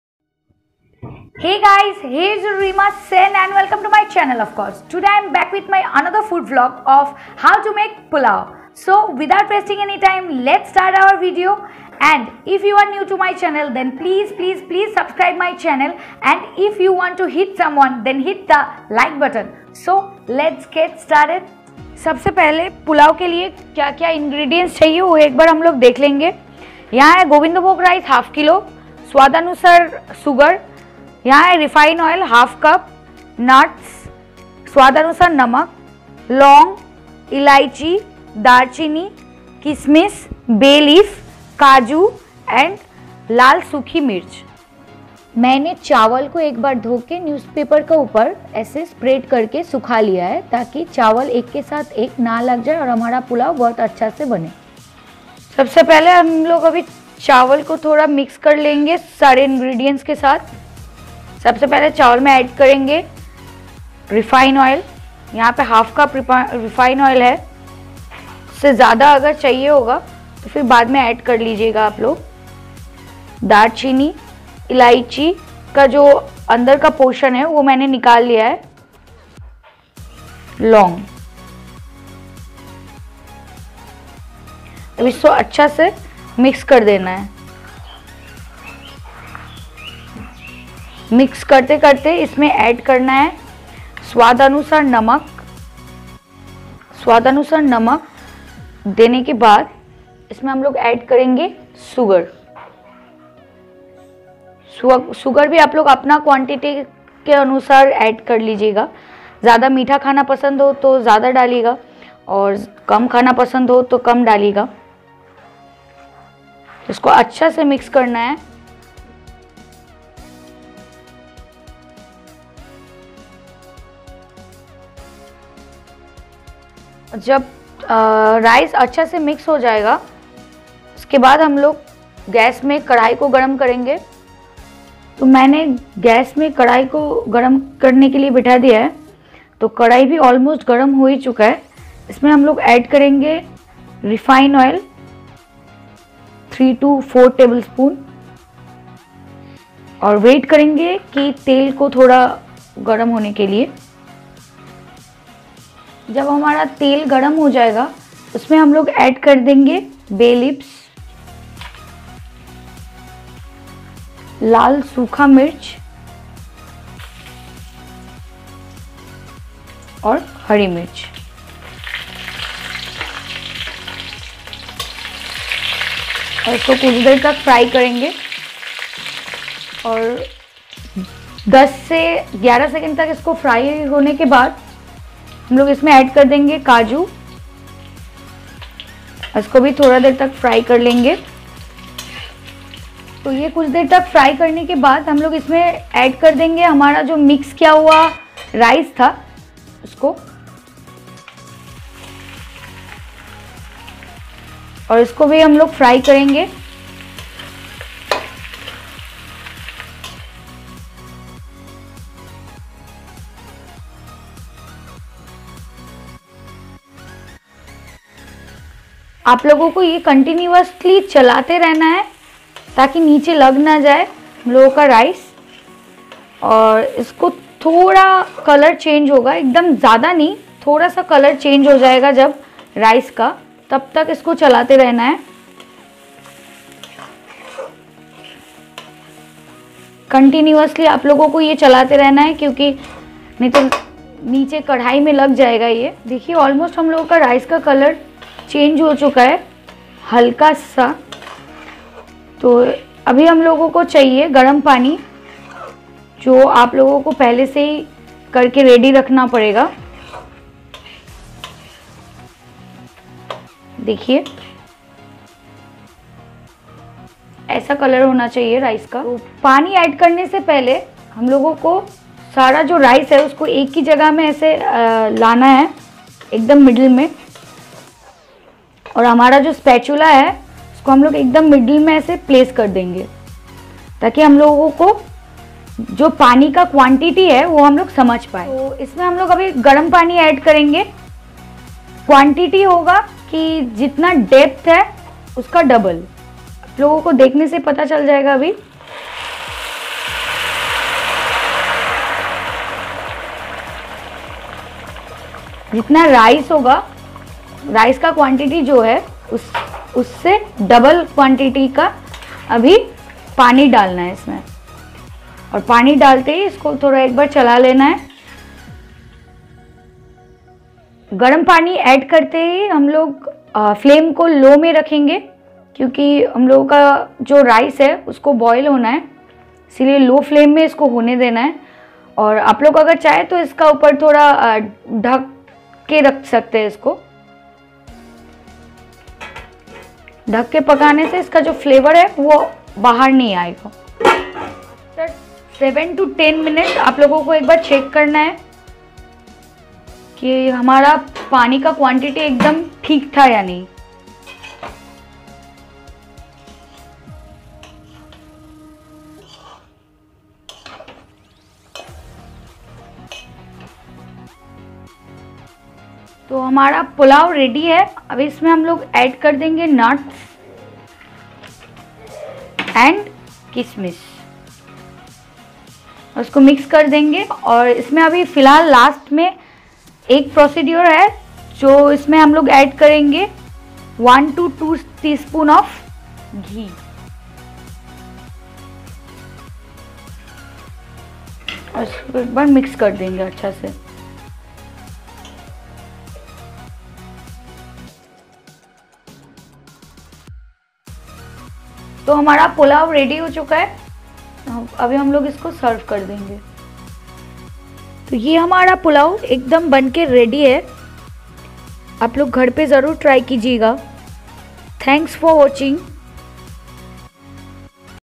Pulao | Pure Veg Pulao |Easy recipe of Pulao | by miss sen
Ingredients:-
Gobindo bhog rice 1/2 kg
Refined oil 1cup
Darchini
Long
Elaichi
Bay leaf
Red dry chilli
Cashew
Nuts
Kishmish
Warm water
Ghee